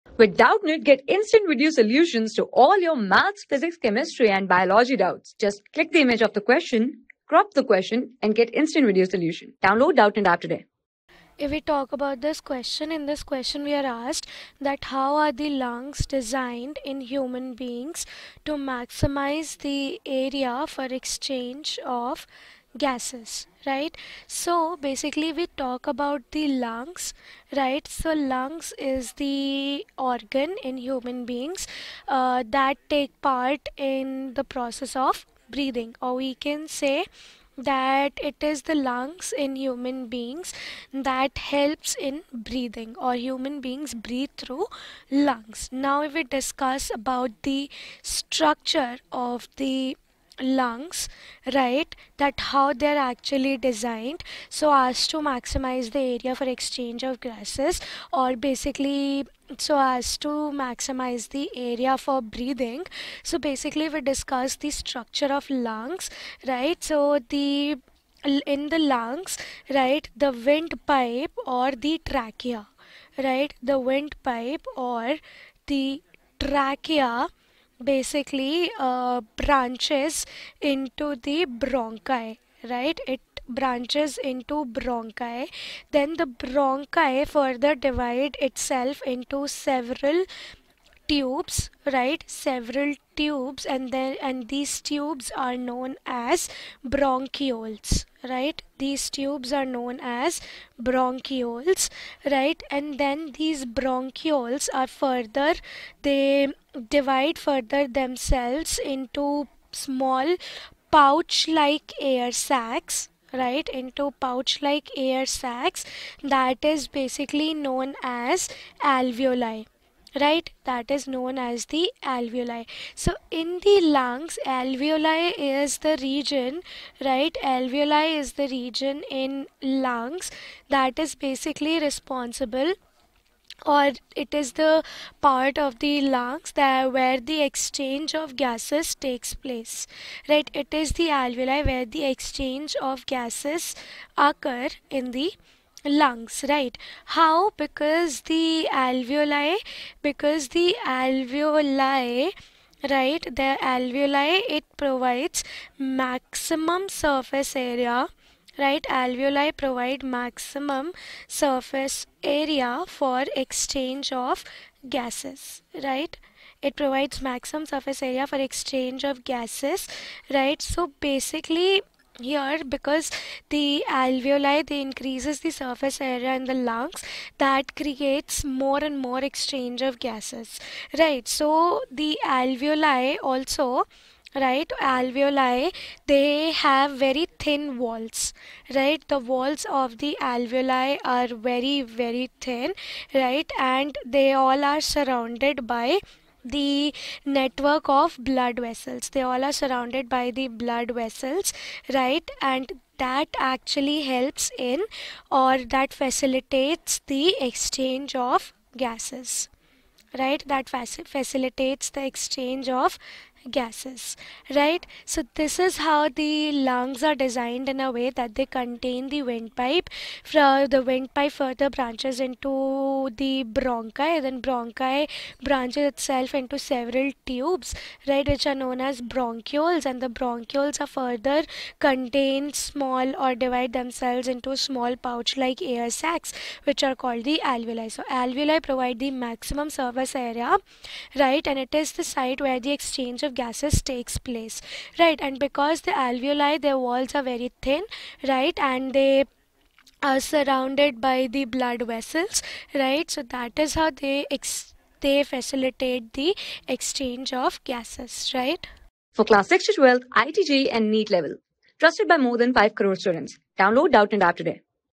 How are the lungs designed in human beings to maximise the area for exchange of gases? | 10 | L...
How are the lungs designed in human beings to maximise the area for exchange of gases?
Class: 10
Subject: BIOLOGY
Chapter: LIFE PROCESSES
Board:CBSE

👉 Have Any Query? Ask Us.